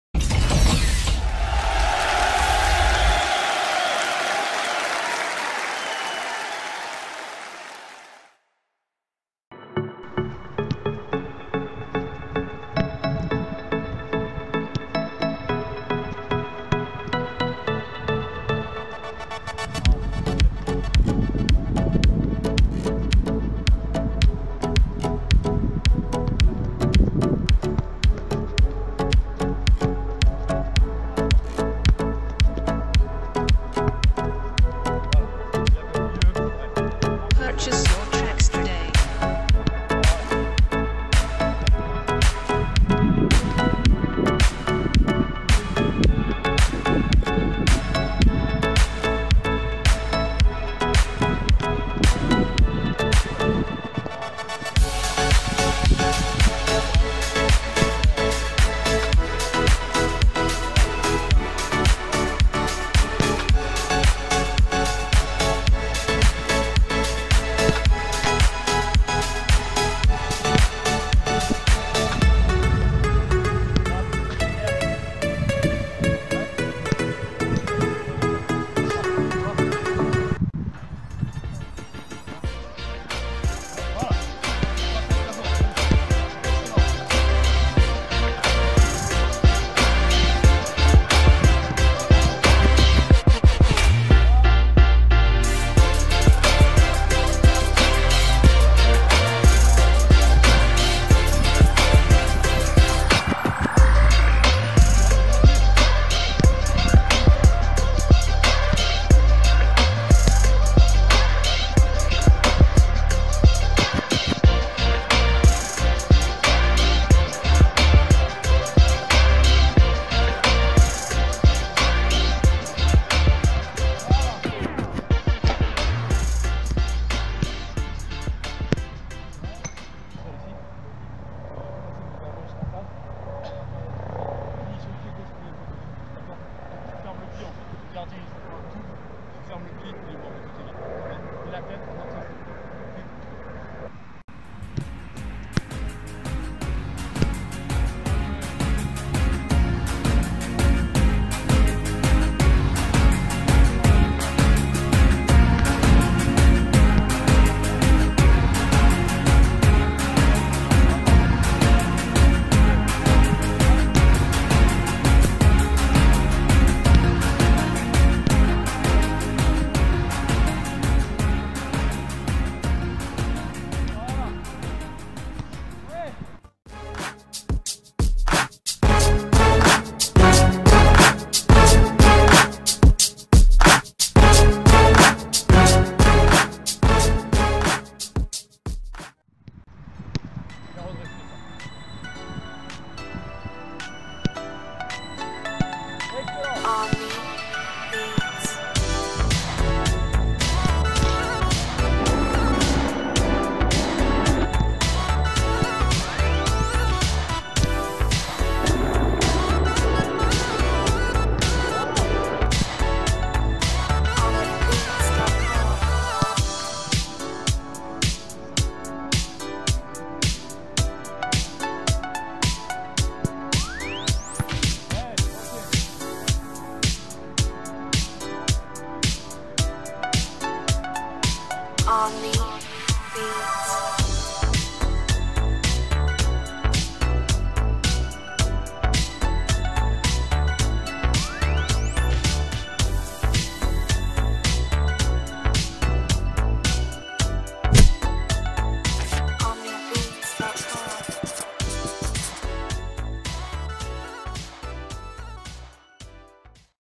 AF Coaching Football : Une séance Spécifique attaquant avec Enzo joueur en CFA à Martigues
AF Coaching Football vous présente de découvrir une séance complète spécifique attaquant avec Enzo Joueur en CFA à  Martigues.
En passant par les différentes phases d'échauffement jusqu'au travail Spécifique de déplacement & finition, découvrez comment les attaquants  sont pris en charge par AF Coaching Football durant une séance.

Contact pour tarifs séances spécifiques attaquants / Milieux Offensifs

SUIVEZ AF COACHING FOOTBALL